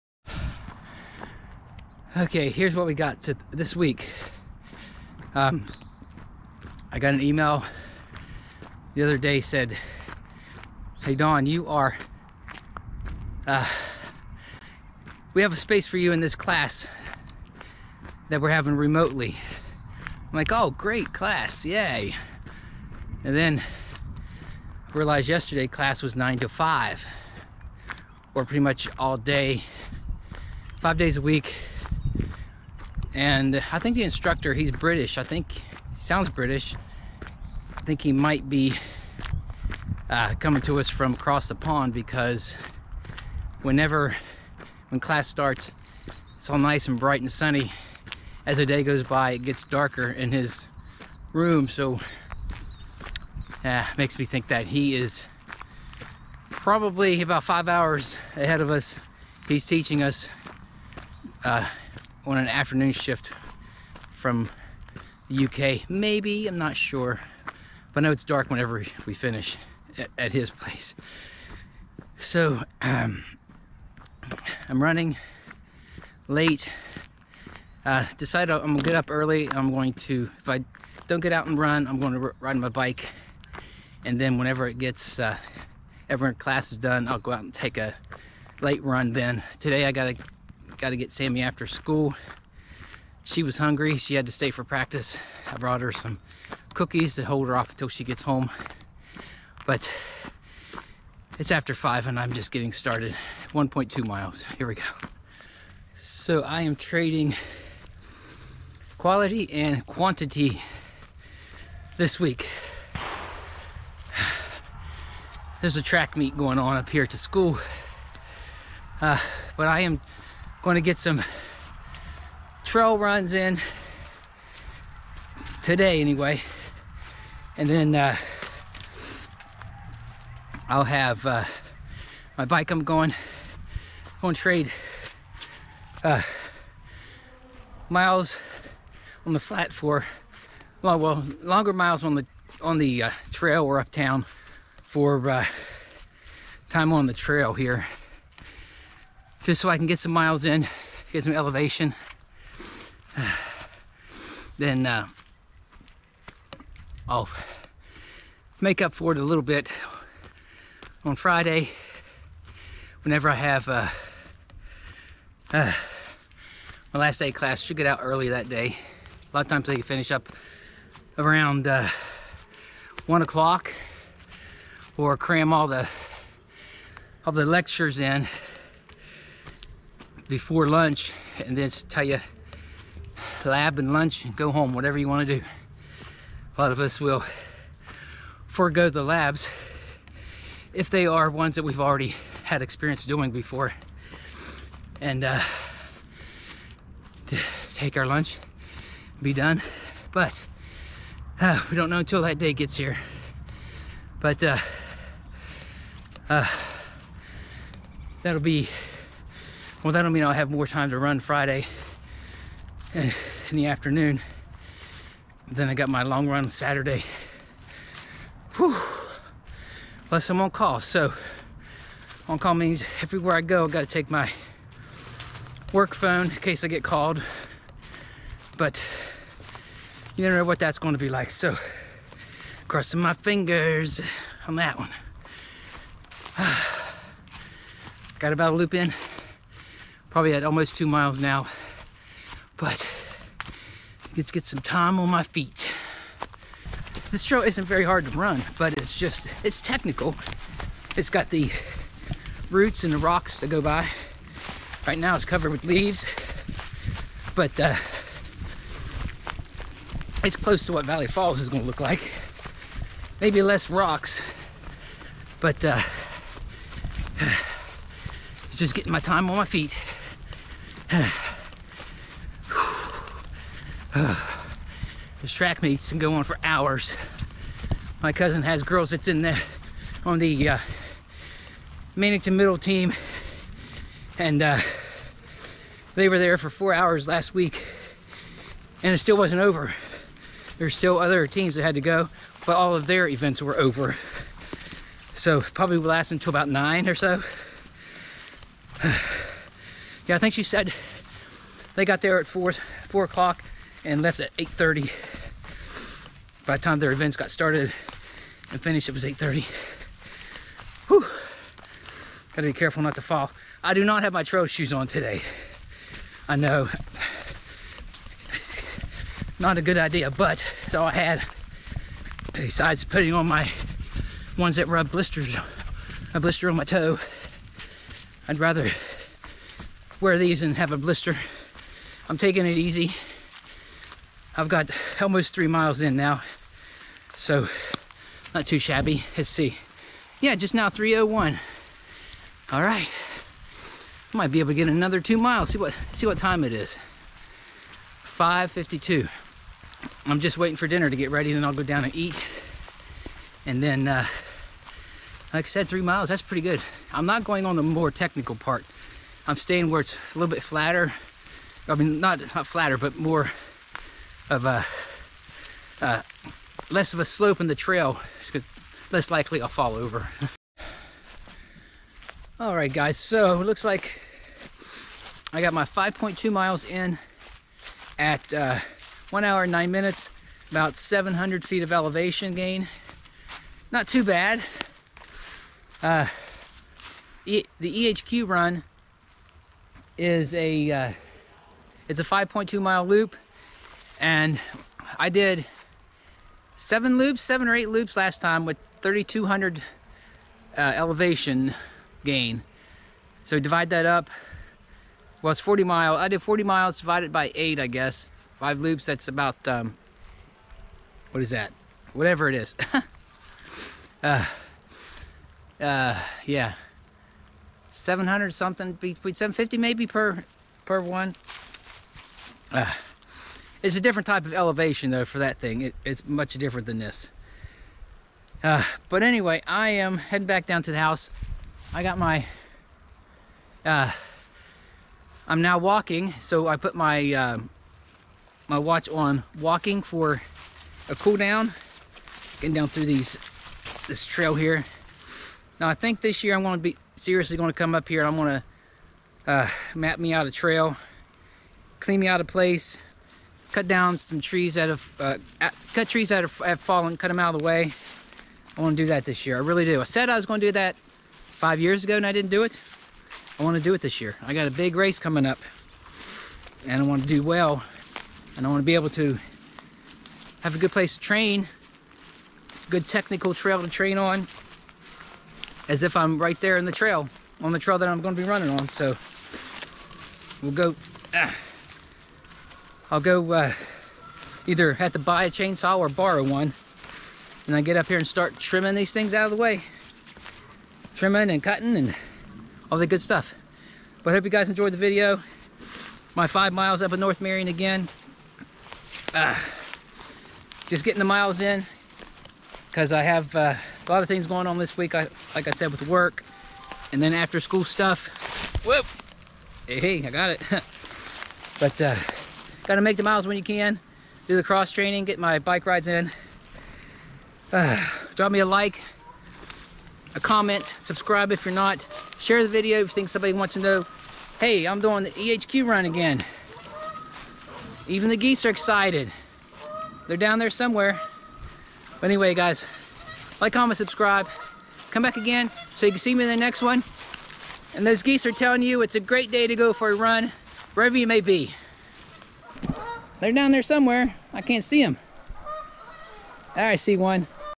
EHQ 12hr Run --- Running lower miles this week
I guess I could call it a taper week but that doesn't make much sense when I will be running longer long run over the next few weeks before my 12hr run.